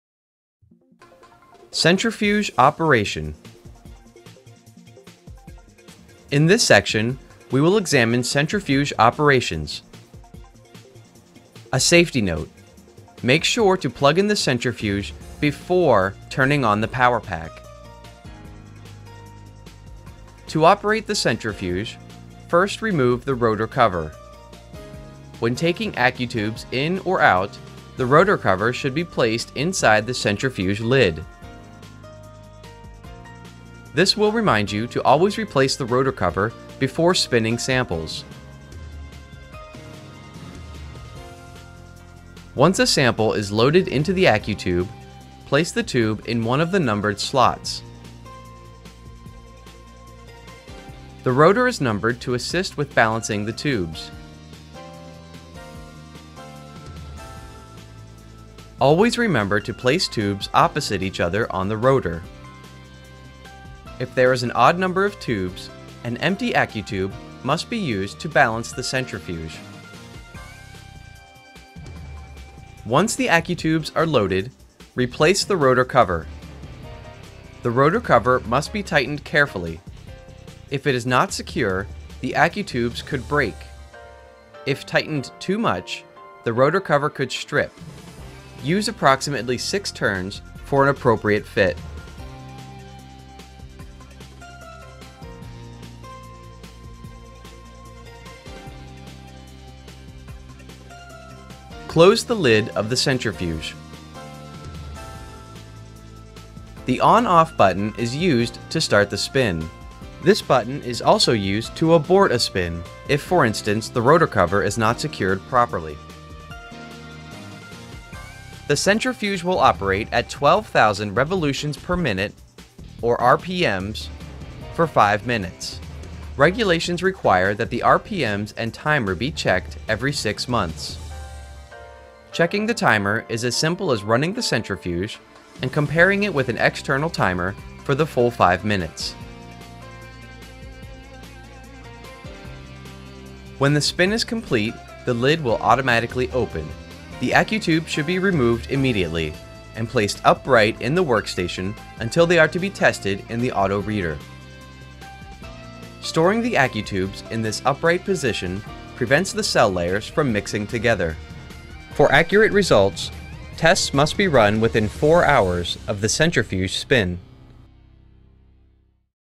QBC AutoRead Plus - Centrifuge Operation
Learn how to load tubes and operate the Centrifuge.